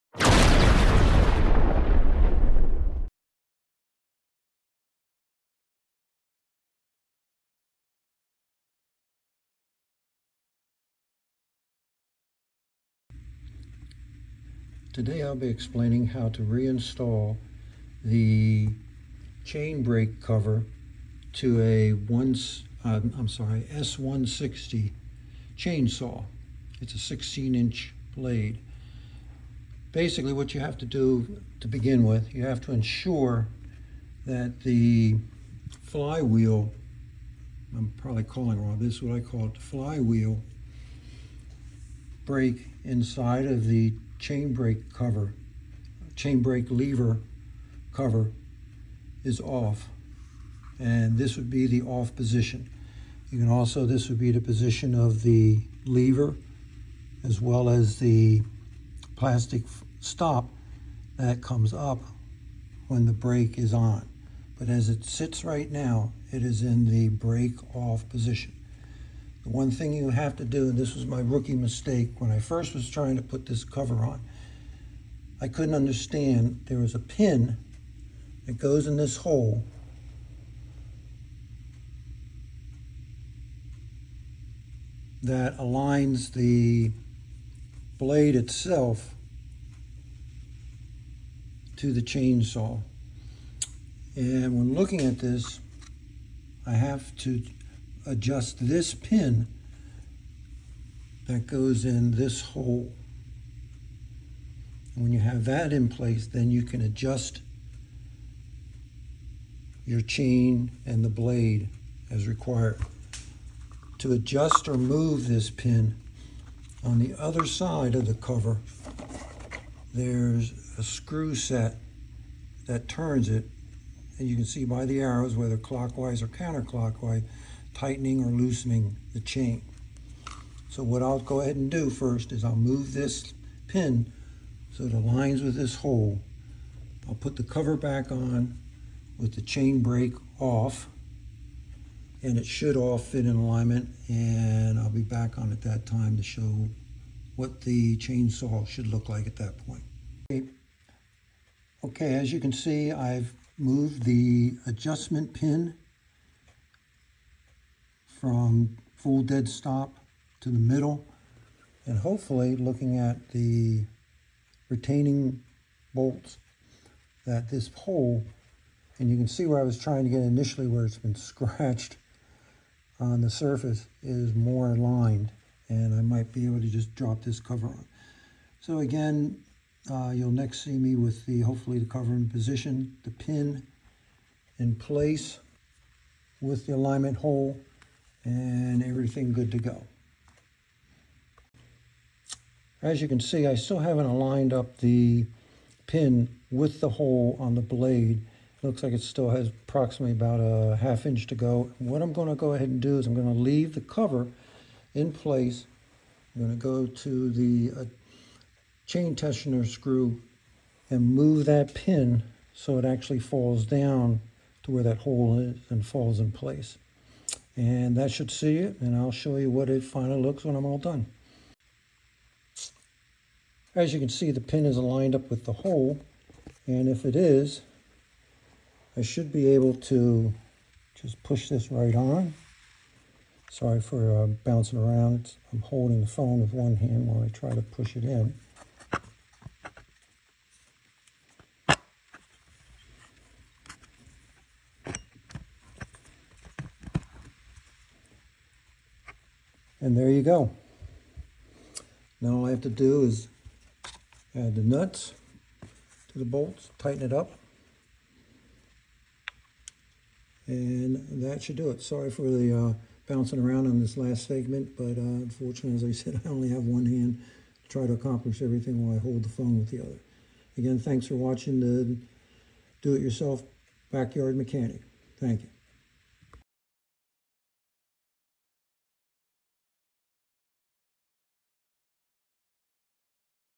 Craftsman 16" Chainsaw S160 - Replacing Brake Handle with Cover
Video explains the ease with replacing the brake handle with cover for a Craftsman 16" chainsaw, Model S160.

DIY Rating: Easy